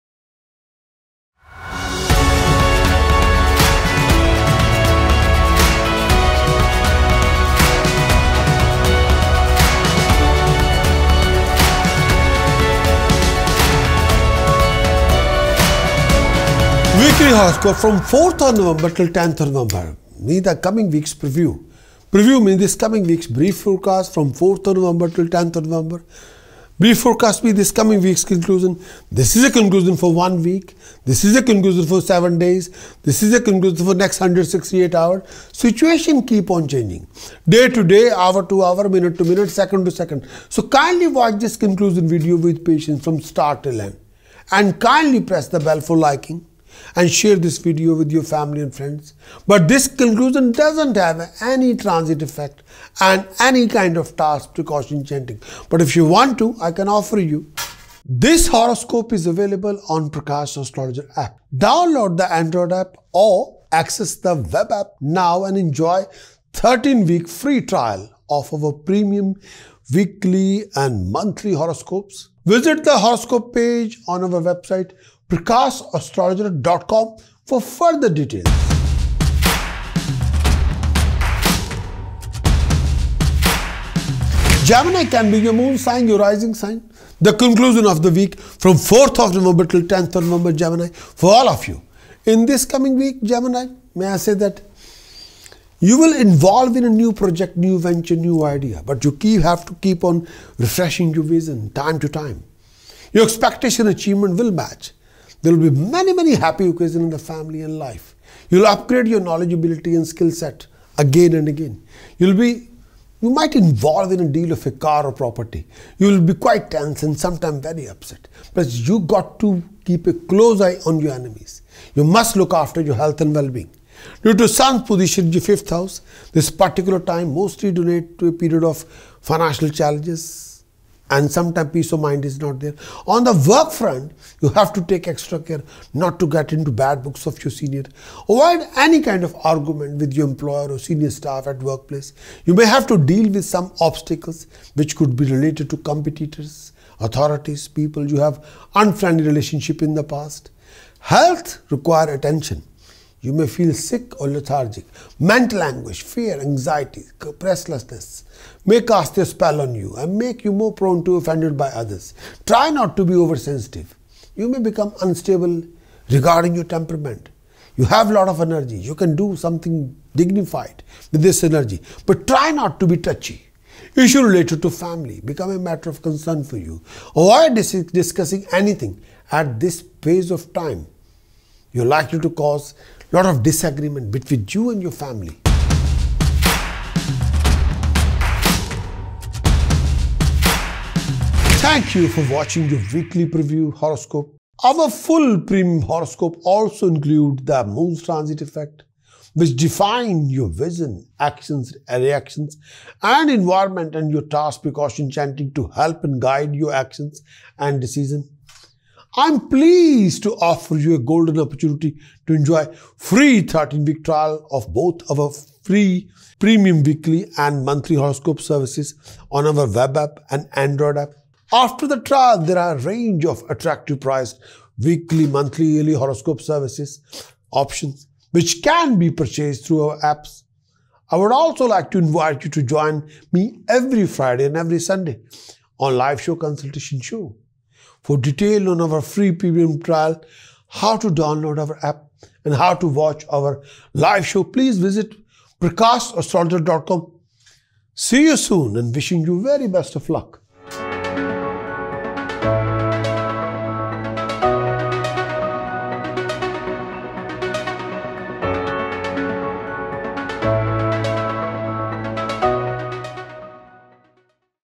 Gemini Weekly Horoscope Video For 4th November 2024 | Preview
Here is your 4th November Weekly Horoscope. In this video we will be covering your health, education, romance, relationship, career, job, profession and conclusion within this Weekly Forecast. Please note that this horoscope video is based on your Moon Sign and does not include Tasks, Precautions and Chanting.